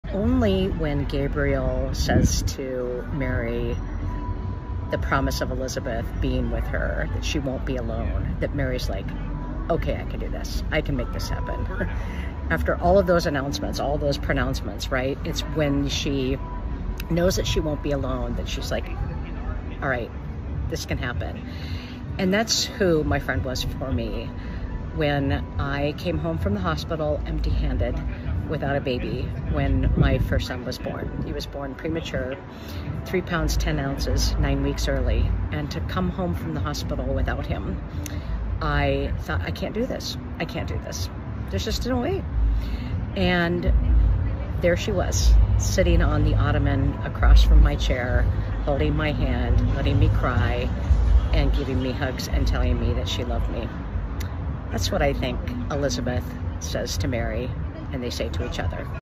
Preacher to Preacher presents the Fourth Sunday of Advent with Karoline Lewis
In Luke 1 Karoline points out the power of friendship. Once Mary knows that Elizabeth is with her in uncertain times, she can accept Gabriel's words.